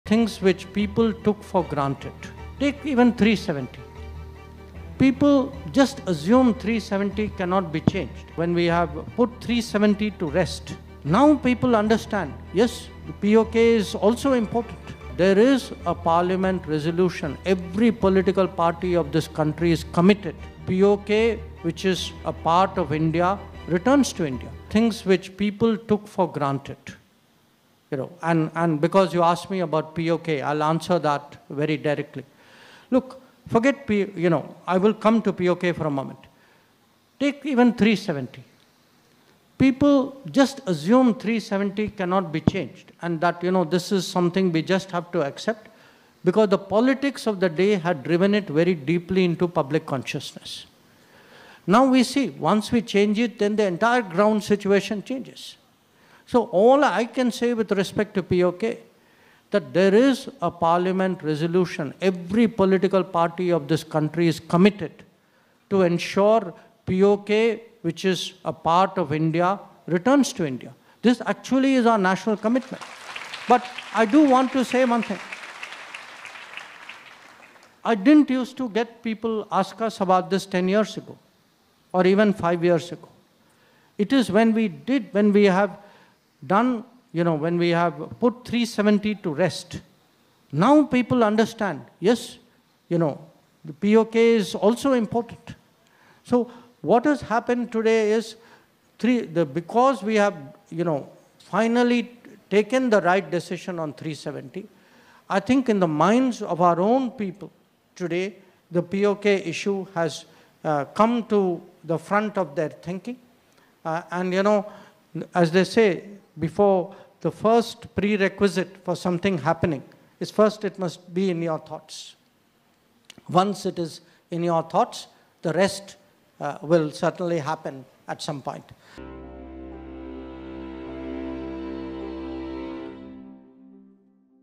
'PoK is a part of India', EAM Jaishankar’s Big Statement on PoK
External Affairs Minister S Jaishankar on Wednesday said that every political party in the country is "committed to ensuring that PoK (Pakistan-Occupied Kashmir), which is a part of India, returns to India," and that "the PoK issue has come to the front of people's thinking" after revocation of Article 370 in Jammu and Kashmir, which was considered a difficult decision.